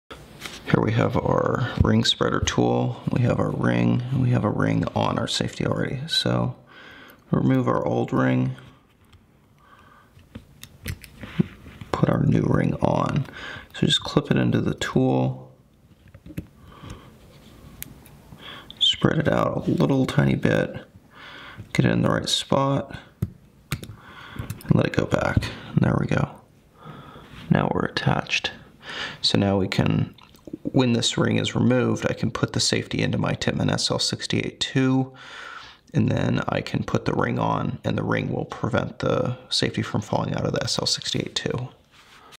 Tippmann SL-68 II Safety C clip Ring
Here is a quick video showing the SL-68 II safety c clip ring removal and reassembly. This ring retains the safety in the body of the SL-68 2.

Items shown in this demonstration include the ring spreader tool (I don't know the technical name), the ring ) and the Safety Pin